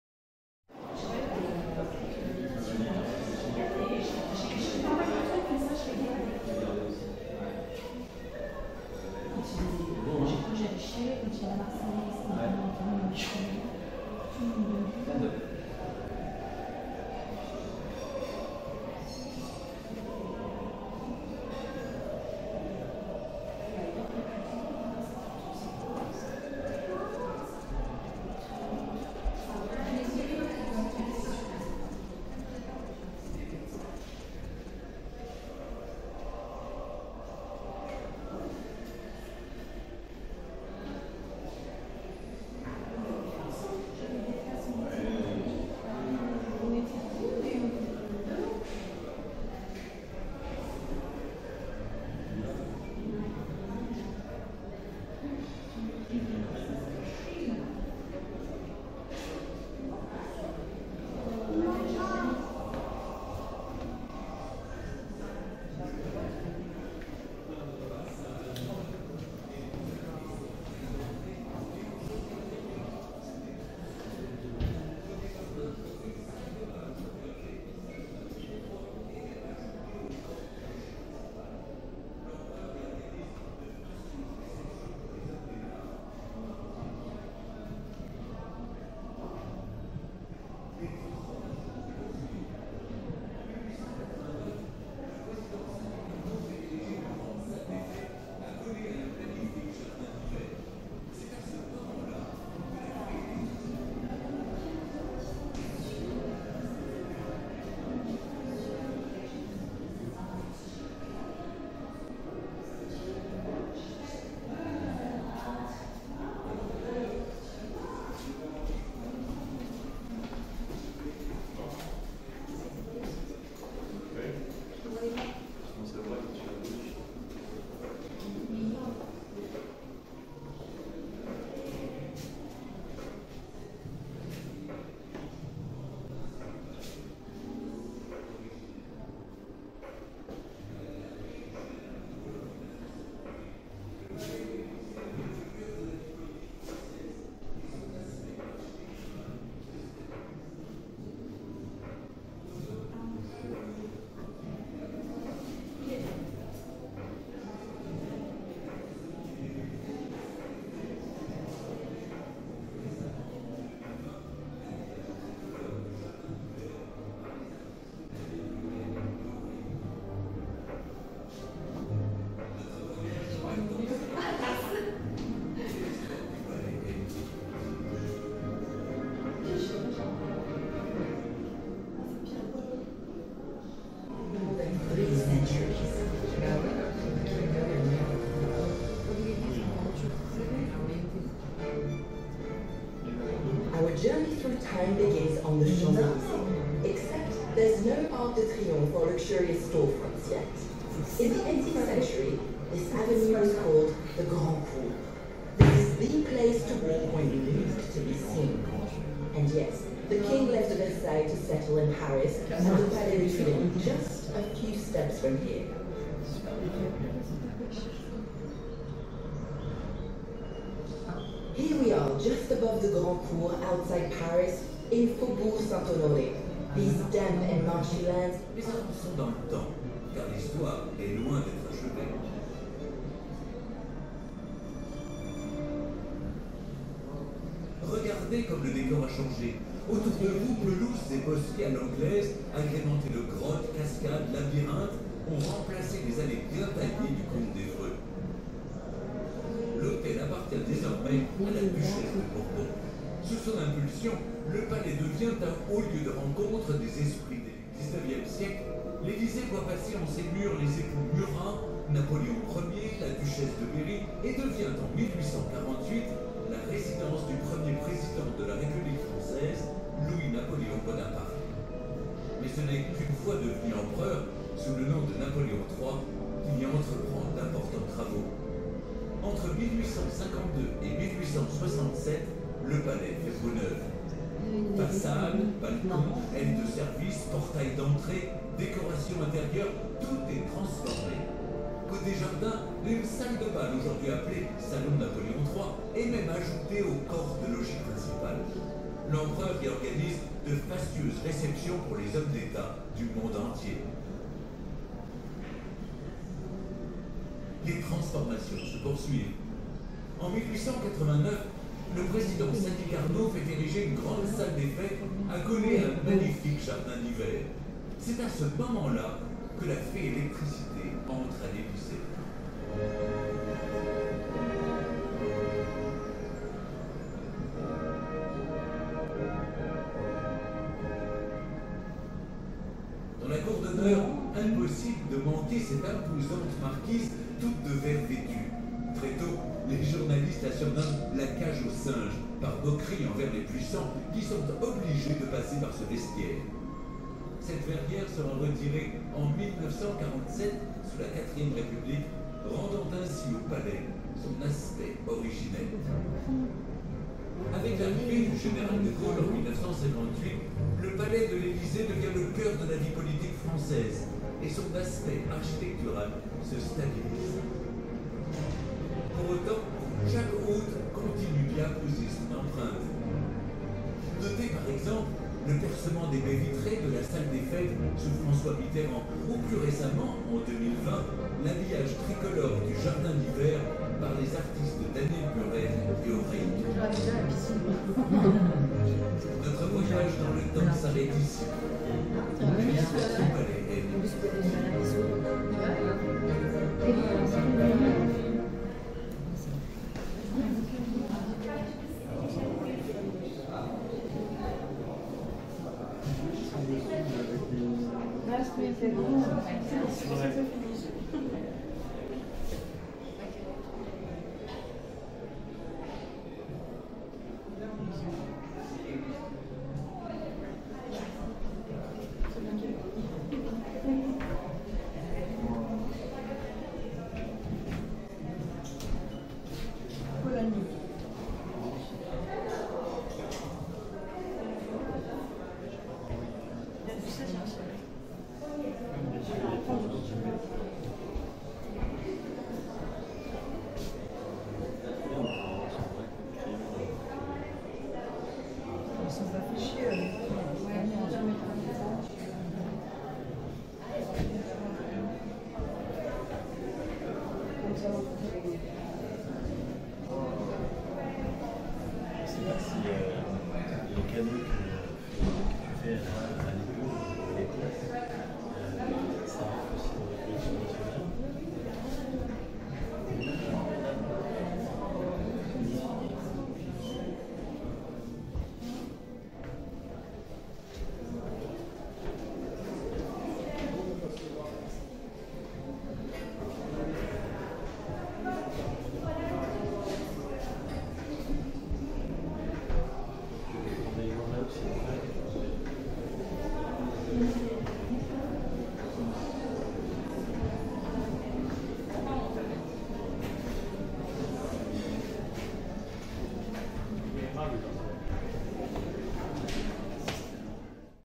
VLOG | Maison Elysée Museum
La Maison Élysée takes a behind-the-scenes look at the palace and the many jobs that keep it running on a daily basis. Through videos and portraits, visitors discover the men and women who contribute on a daily basis to the running of this place. From housekeeping to the Republican Guard, each function is illustrated to reveal the extraordinary diversity and commitment of the teams working at the Élysée Palace.

The cabinet of curiosities is a veritable cabinet of curiosities displaying a collection of diplomatic gifts given and received by President Emmanuel Macron over the years. Each gift tells a unique story. Whether an artwork, a shared skill or an emblematic souvenir, these items illustrate the subtle art of diplomacy. They embody the cultural exchanges and links forged between France and other nations, reflecting the values of friendship and mutual cooperation.